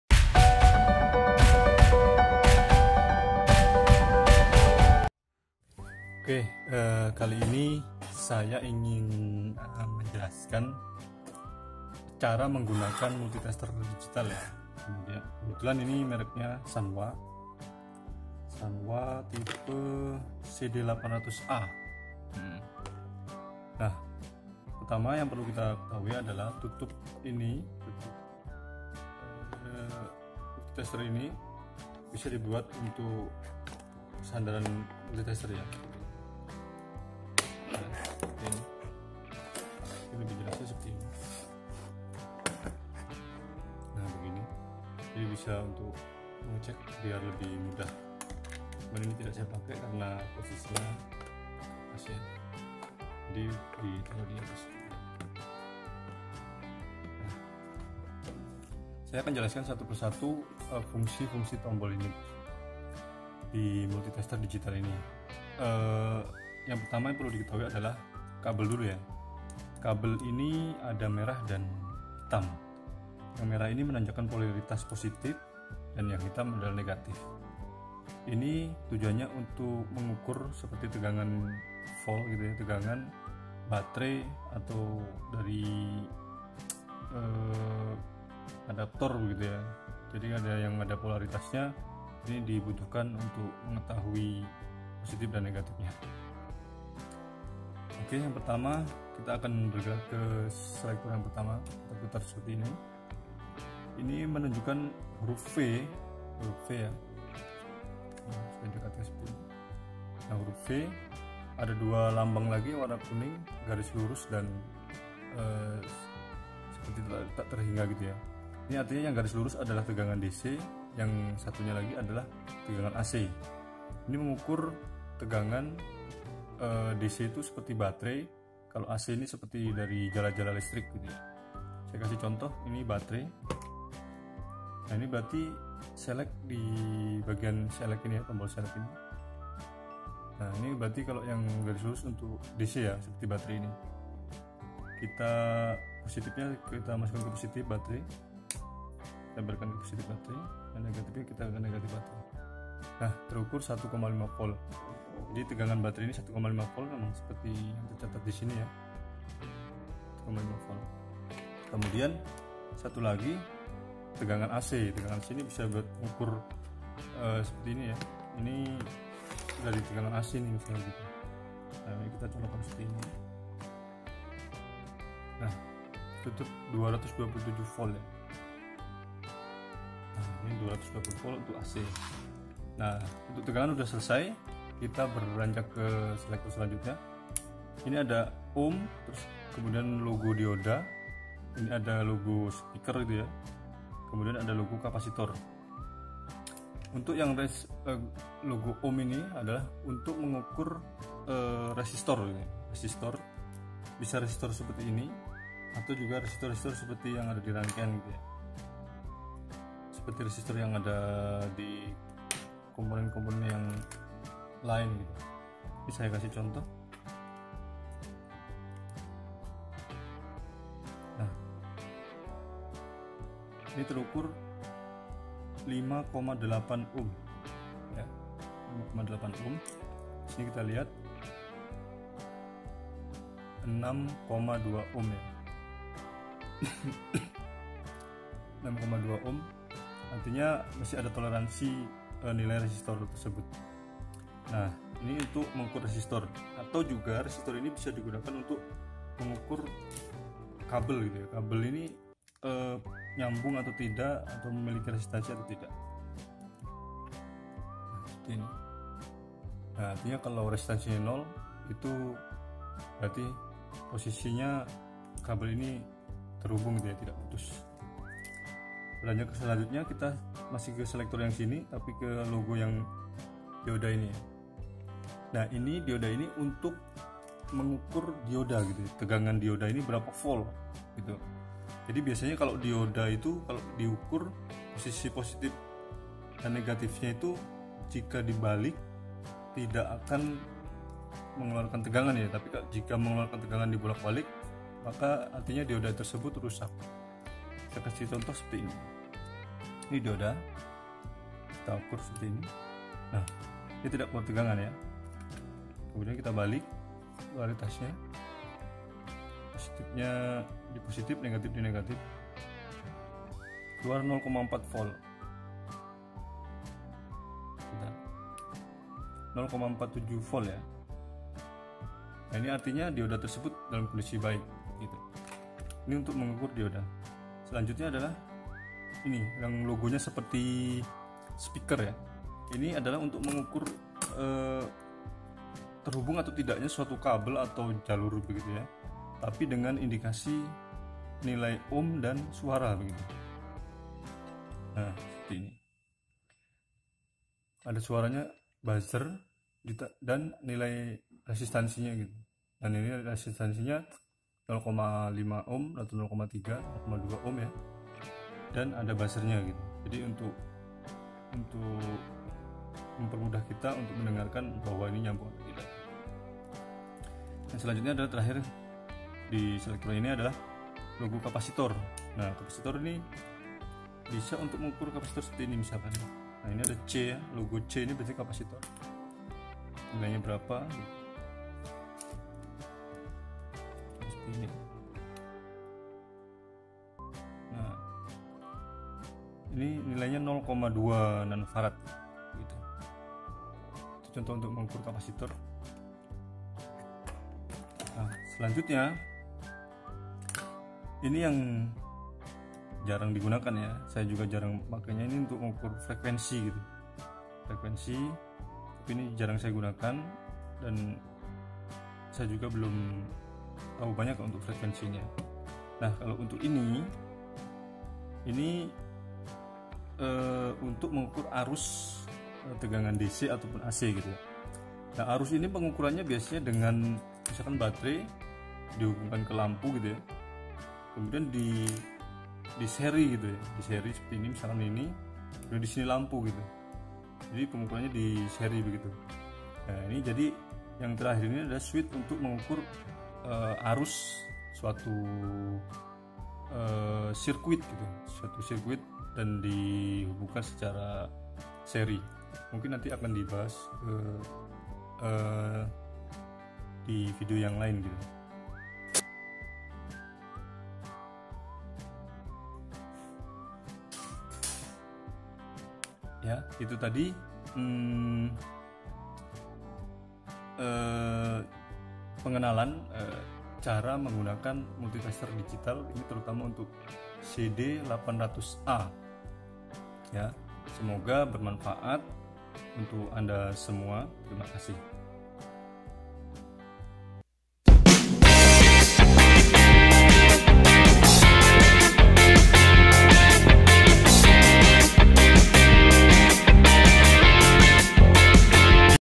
Tutorial Lengkap Cara Menggunakan Multitester Digital [CC]
Tutorial Lengkap Cara Menggunakan Multitester Digital
Dalam Video Ini Dijelaskan Secara Lengkap dan Jelas cara menggunakan multitester digital. Multitester atau multimeter digital yang digunakan dalam video ini adalah Sanwa CD800a. Dalam Video ini dijelaskan secara detail fungsi dari masing-masing tombol yang sering kita gunakan. Semoga dapat membantu anda.